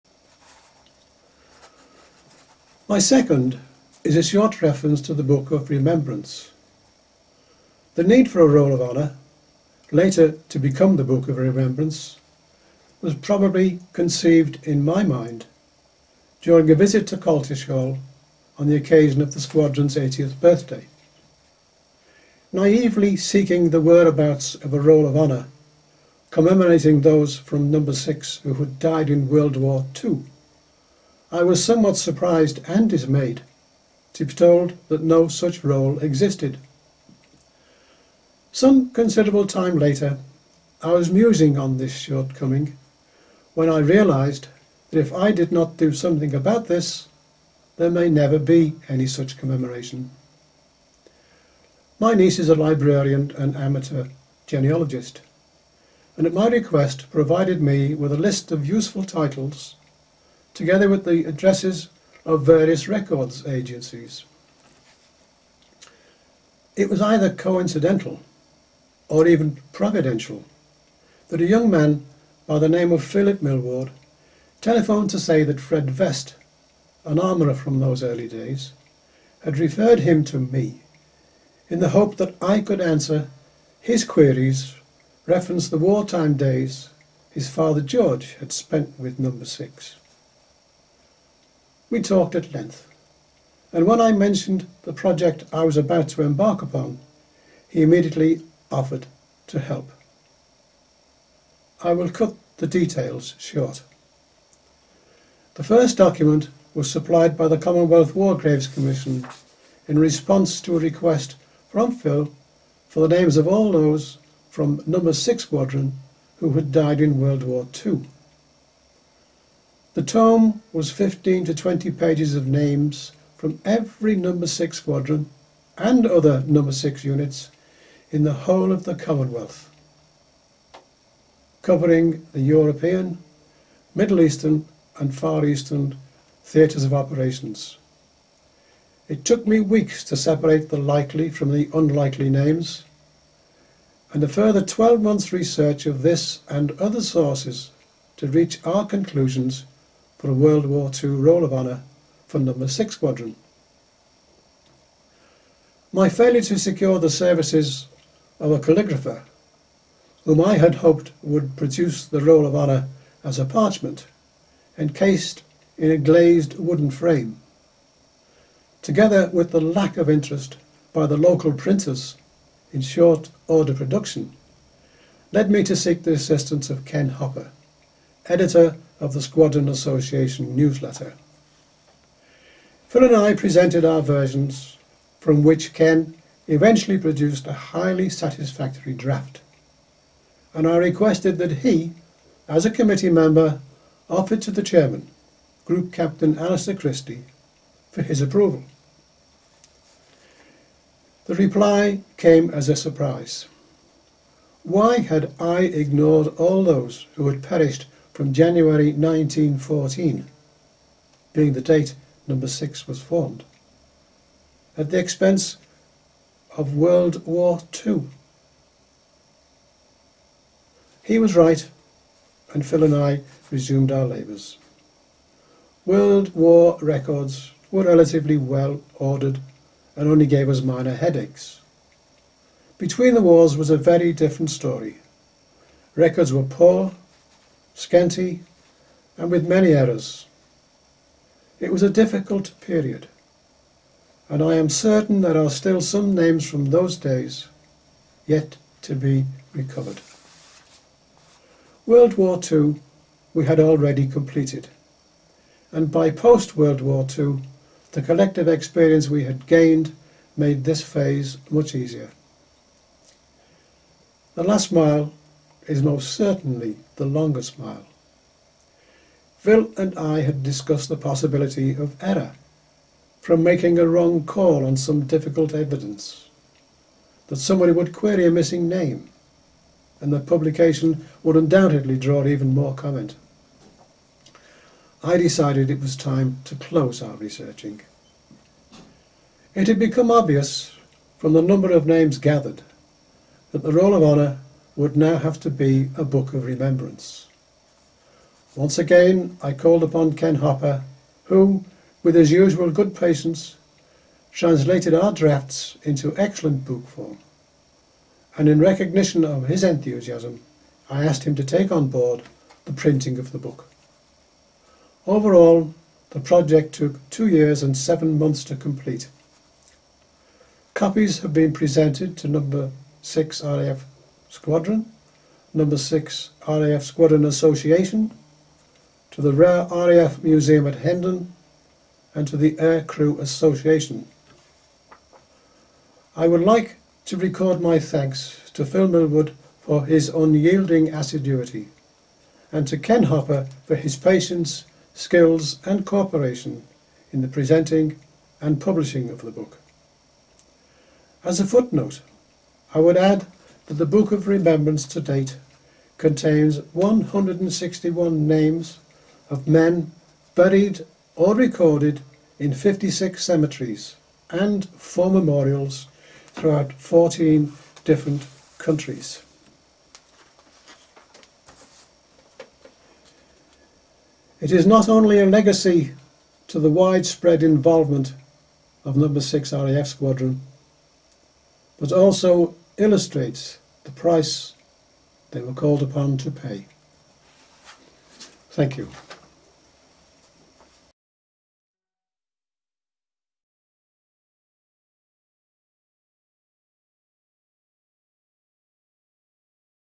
Harry Lundberg on the Book of Remembrance
Harry tells the story about the creation of  Book of Remembrance for all those Personnel who died in the Service of 6 Squadron RAF